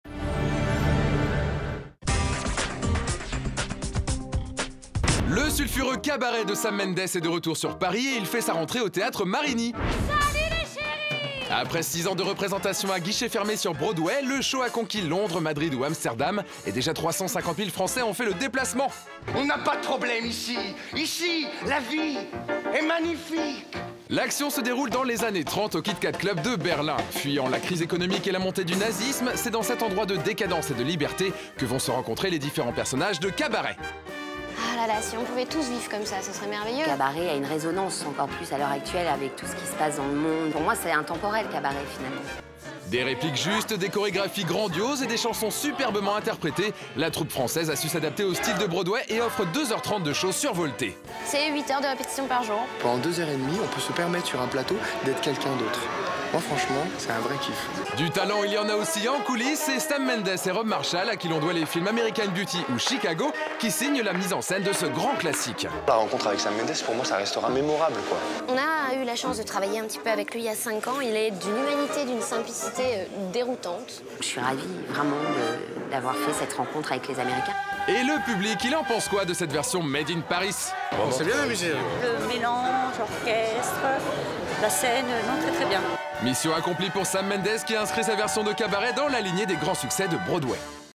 Cabaret Le Musical - "Sortie d'Artiste"
Après avoir accueilli 350 000 spectateurs au cours de 450 représentations aux Folies Bergère d'octobre 2006 à janvier 2008, le célèbre musical CABARET, qui avait marqué les esprits du public à Paris et de la profession avec 7 nominations aux Molières, est de retour au Théâtre Marigny depuis le 6 octobre 2011 puis en tournée en France dès janvier 2012.